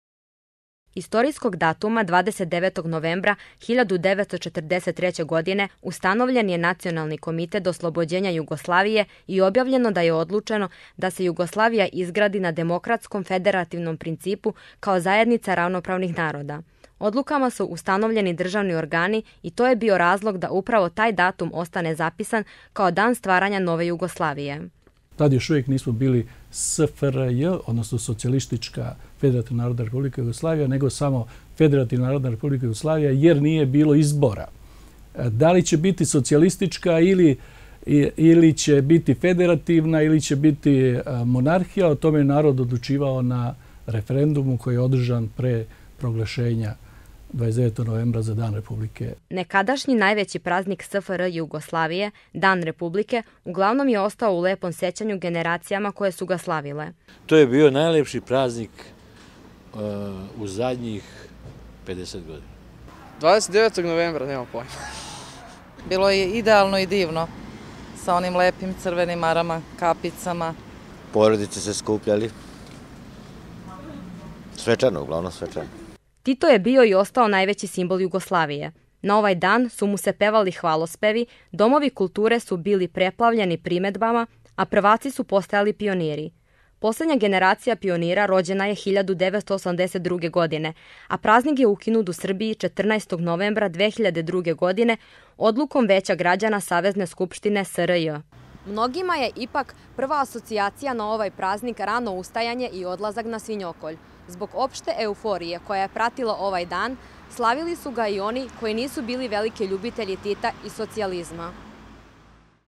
Dan republike
Dan Republike u bivšoj SFRJ slavio se u znak sećanja na 29. novembar 1943. kada je u Jajcu doneta doneta odluka o formiranju države federalnog ustrojstva, Jugoslavije. Praznik se decenijama proslavljao dva neradna dana i bio je povod za slavlje, igranke, druženja, školske priredbe, a prvaci su dobijali crvene marame.